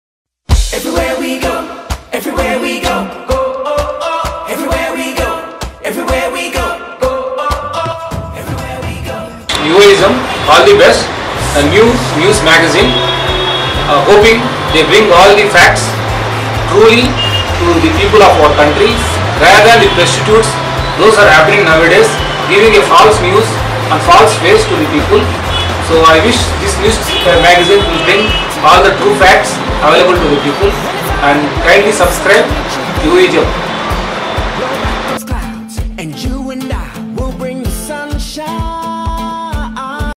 Dr.C.Vasu Reddy, VR Medical Surgical & Cancer Care wishing all the best to YUVAISM NEWS
Kurnool People wishing all the best to YUVAISM NEWS & MAGAZINE